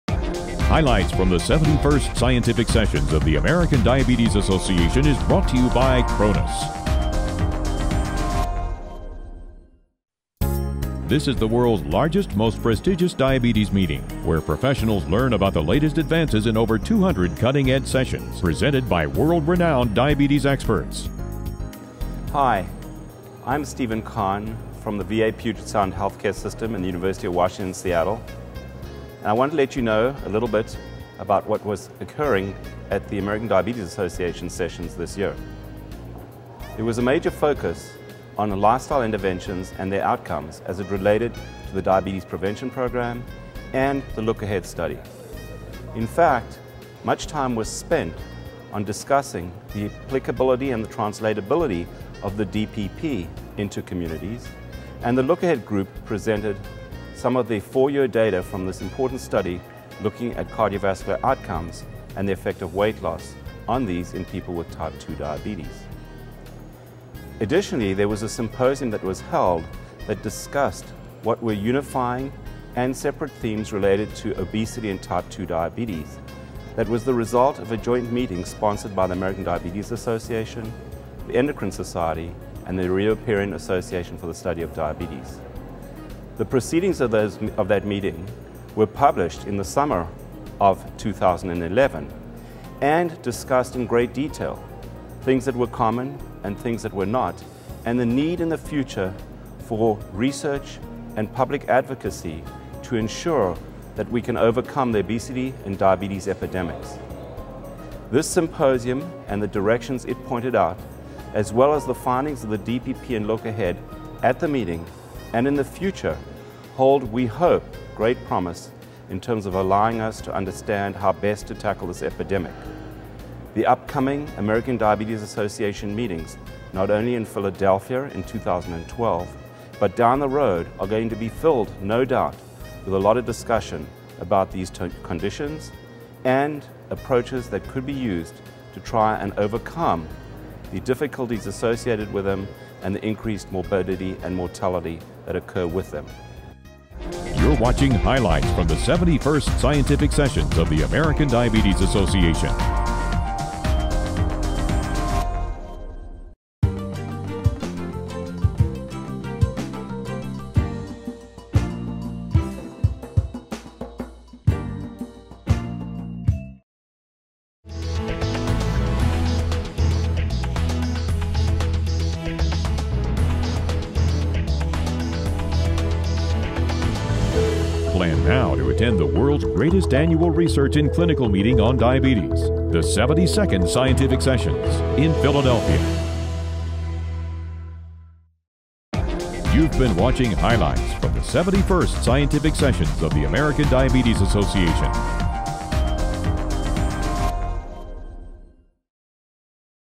Video Highlights of the 71st Scientific Sessions from San Diego - Sunday
Steven E. Kahn, MD (VA, Puget Sound Health Care System) reports on the future directions of treatment for diabetes and obesity as presented during the 71st Scientific Sessions in San Diego.